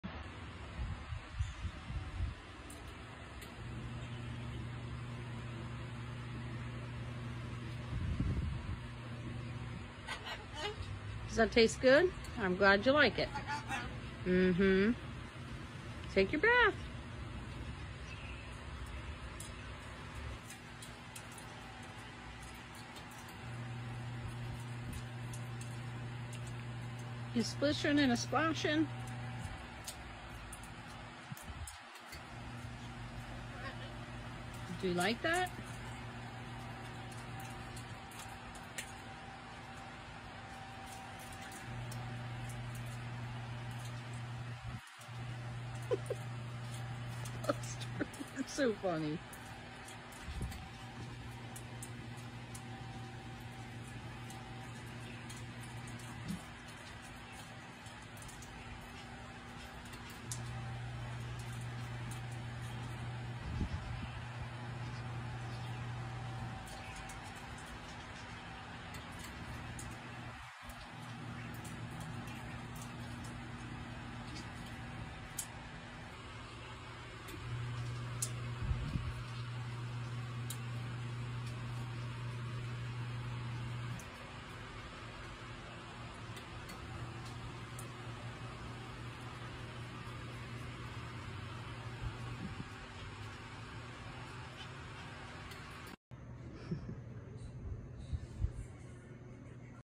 Umbrella cockatoo bath time in his aviary & dinner with Dad
Buster is so funny. Of course no evening is complete without dinner with Dad.